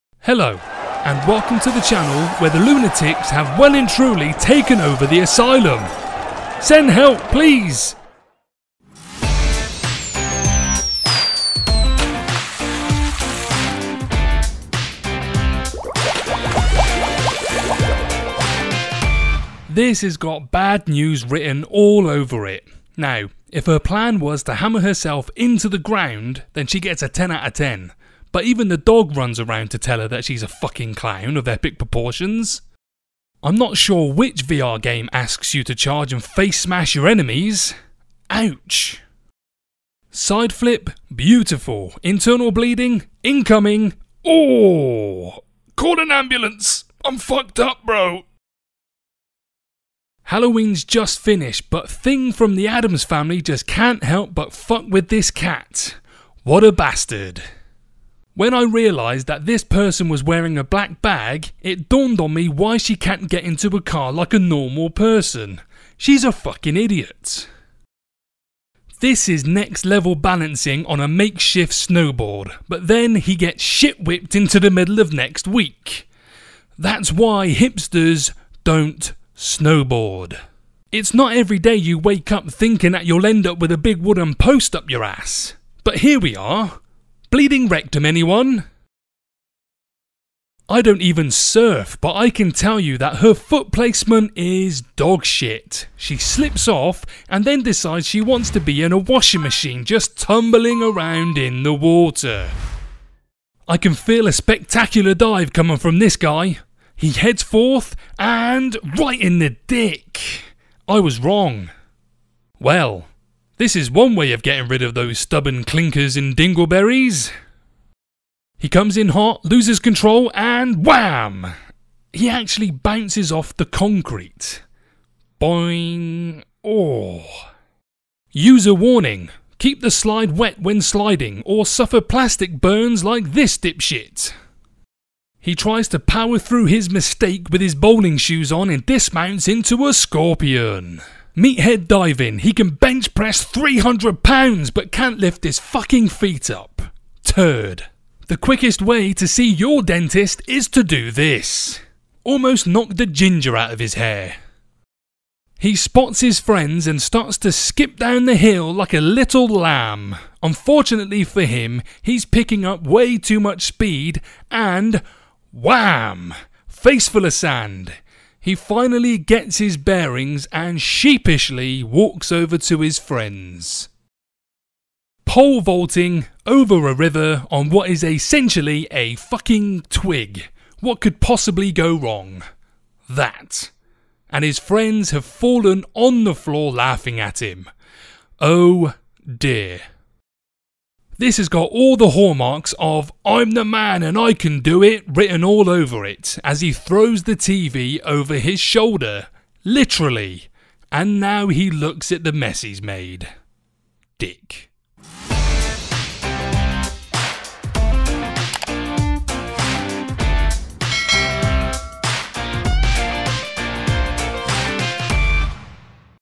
Fail Mashup Vol 10 | VR FAIL | NUTCRACKERS & Trampoline Twits
Welcome to Volume 10 of the Fail Mashup. We have some very funny, but painful fails which shows how silly some of humanity can be.

Enjoy seeing people make a fool of themselves with a hilarious commentary during this fail mashup compilation video. For all of that and more watch the entire video, if you have then check out the videos below for more laughs.